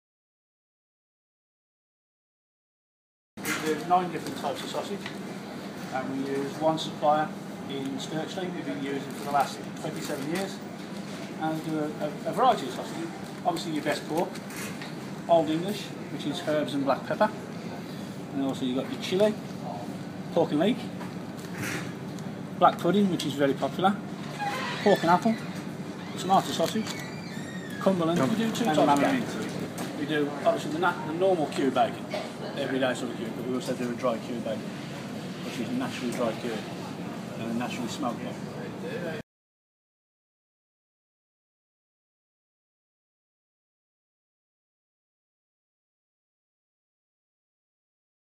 National Butchers Week - Becketts Farm talks sausage and bacon
Becketts Farm butcher Ken Polden talks us through Becketts Farm 9 sausage varieties and dry cured bacon.